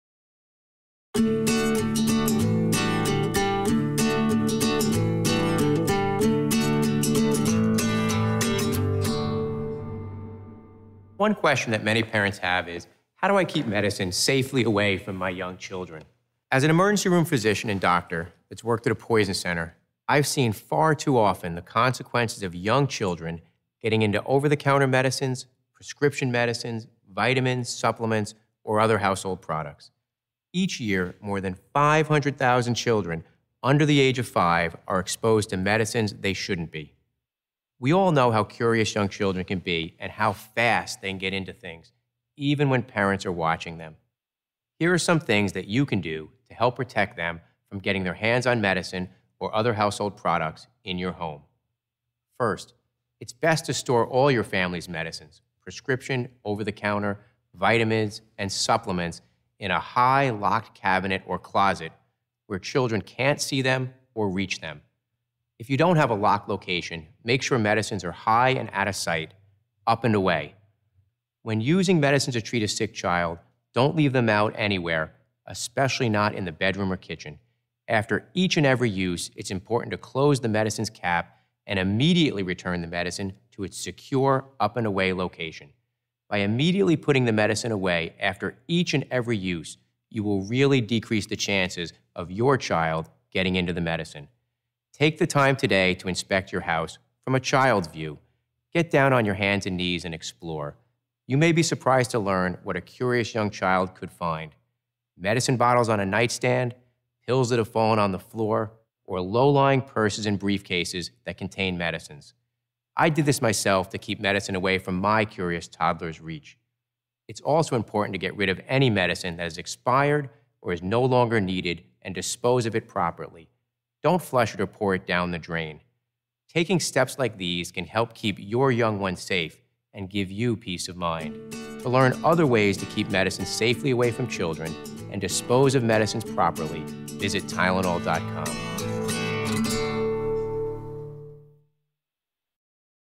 How Can I Store Medicine Safely Away From Children?
Learn about medicine storage and how to store your medicine securely from children. Hear Dr. Ed Kuffner talk about the dangers of accidental poisoning in children under the age of five.